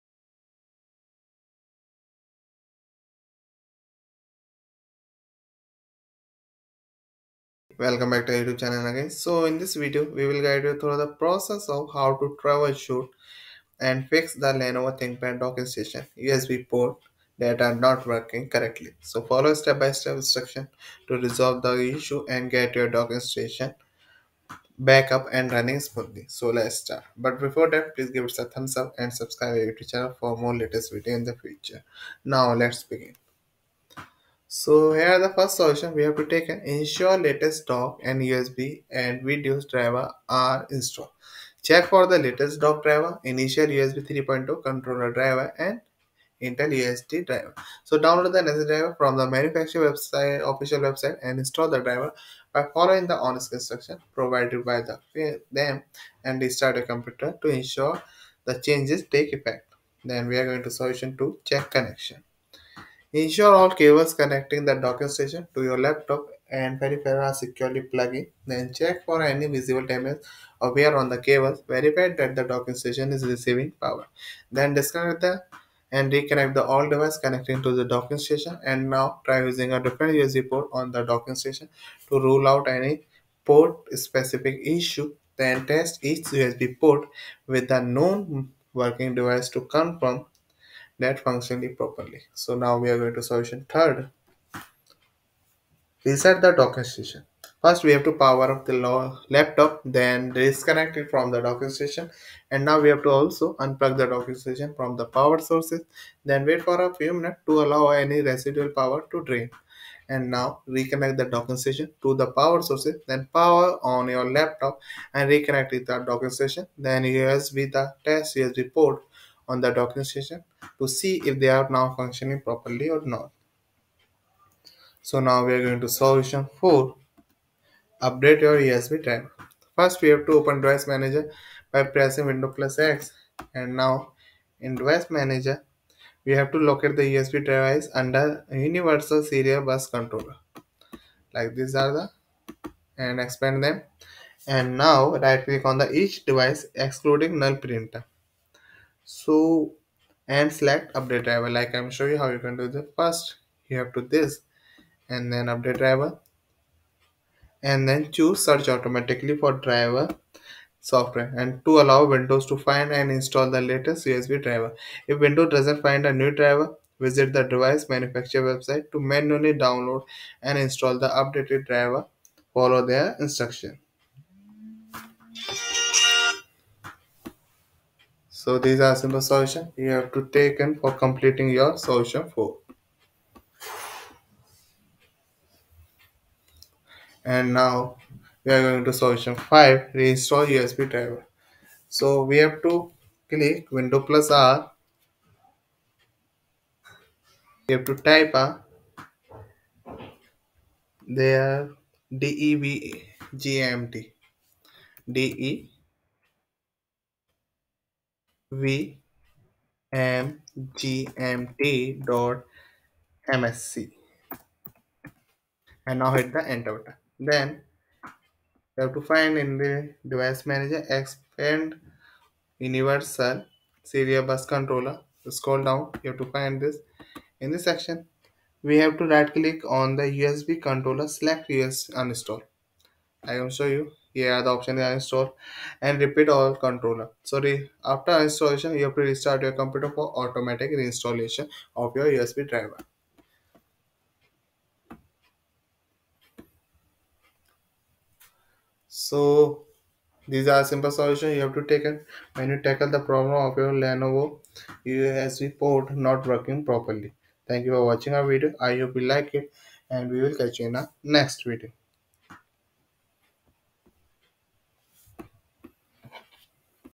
How To Fix Lenovo Thinkpad Docking Station USB Ports Not Working
Discover effective solutions for resolving the issue of malfunctioning USB ports on your Lenovo Thinkpad Docking Station in this informative video. Follow our step-by-step guide to easily fix this problem.

0:35 Solution 1: Ensure Latest Dock, USB, and Video Drivers are Installed
1:02 Solution 2: Check Connections
1:33 Solution 3: Reset the Docking Station
2:04 Solution 4: Update USB Drivers
3:10 Solution 5: Reinstall USB Drivers